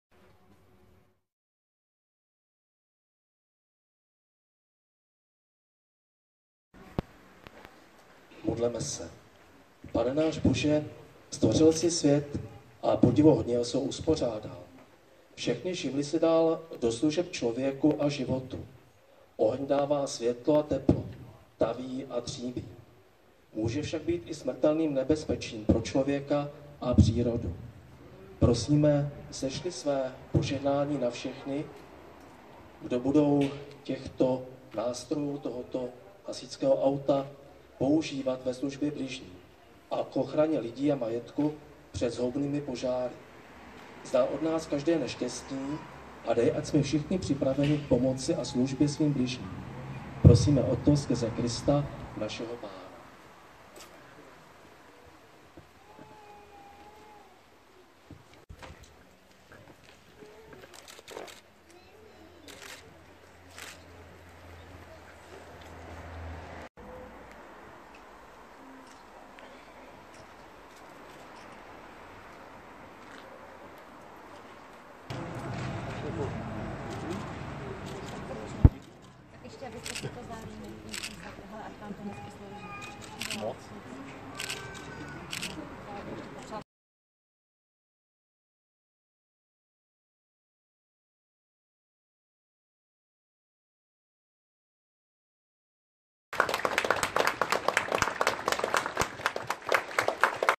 Křest nové cisterny SDH Jilemnice
Hasiči v Jilemnici (Výjezdová jednotka JPO II) mají novou cisternu  CAS 20 4000/240 S2R na podvozku Scania P 480.